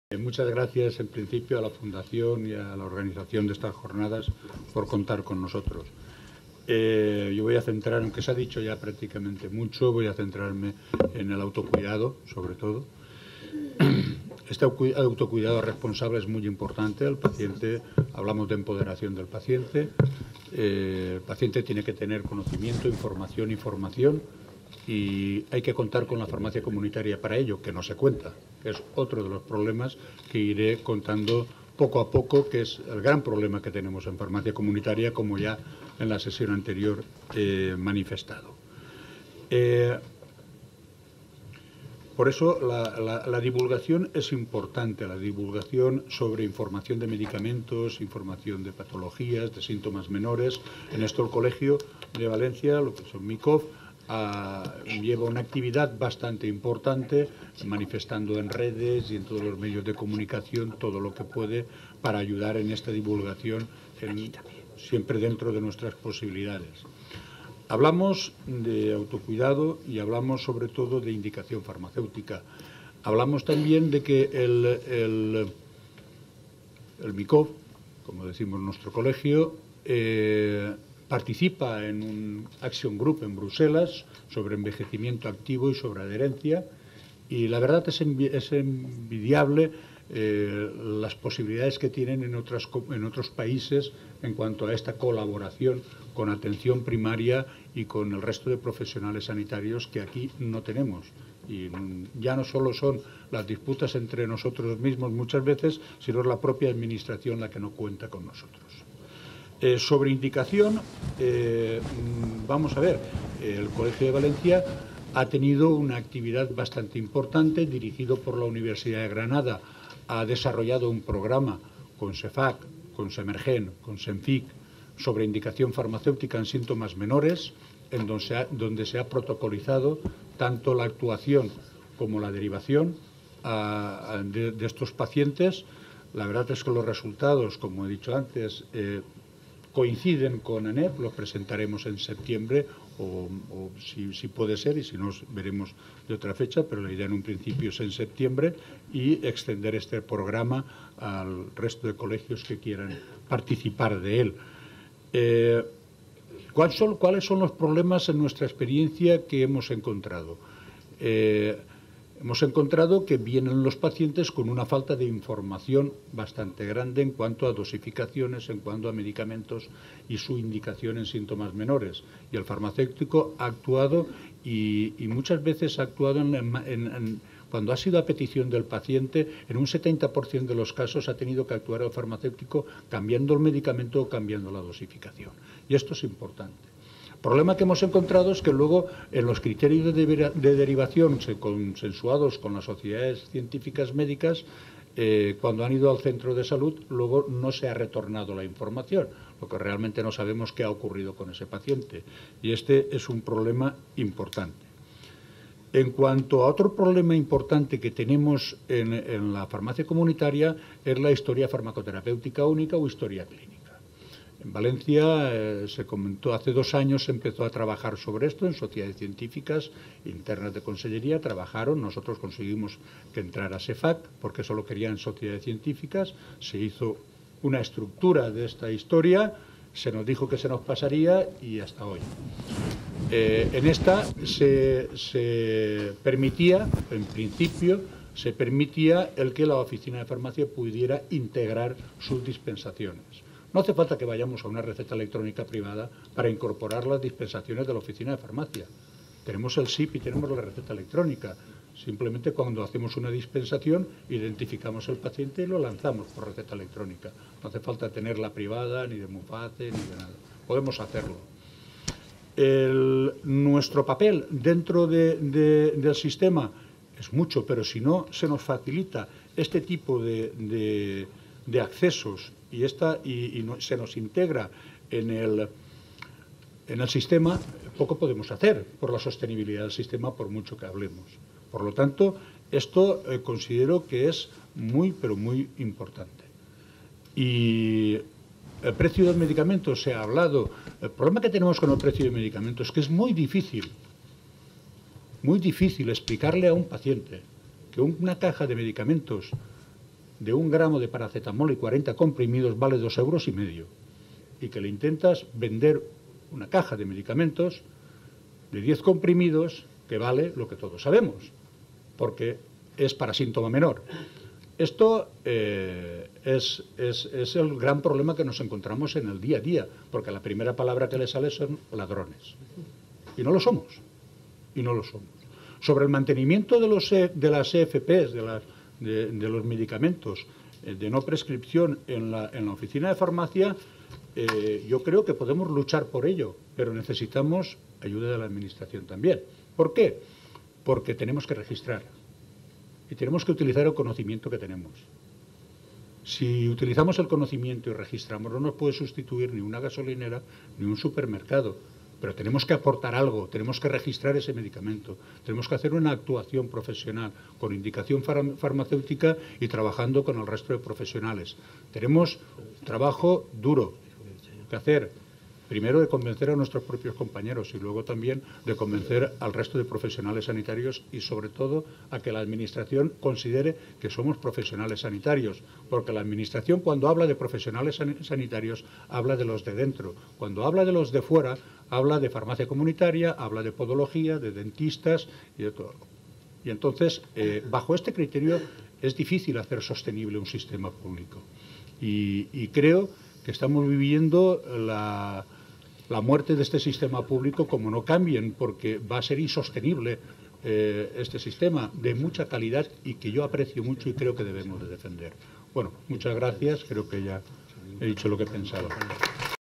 36) SESIÓN DE DEBATE - Moderación por D. Ignacio Para Rodríguez-Santana
D. Ignacio Para Rodríguez-Santana, Presidente de la Fundación Bamberg

EL AUTOCUIDADO DE LA SALUD, imprescindible para el Sistema Sanitario, tomó lugar el día 4 de julio de 2019 en el CONSEJO GENERAL DE COLEGIOS OFICIALES DE FARMACÉUTICOS, Madrid.
El acto ha sido organizado, moderado y gestinado por la Fundación Bamberg.